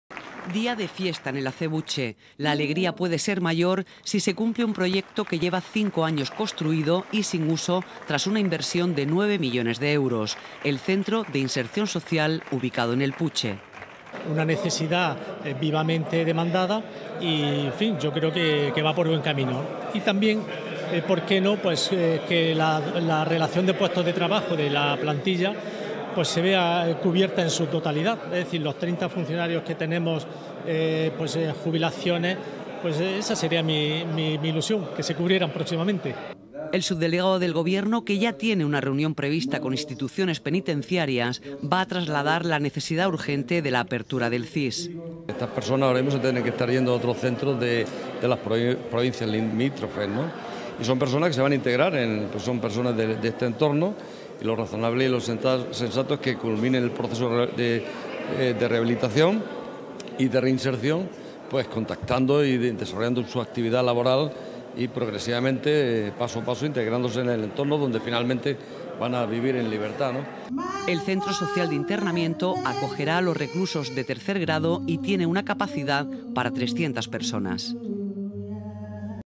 Aplicar el tercer grado antes de final de año
La prisión de El Acebuche tiene preparado un centro de inserción social para poder llevar aplicar el tercer grado desde hace seis años, pero sin funcionar. Desde la dirección de El Acebuche dicen que puede empezar a funcionar antes de final de año.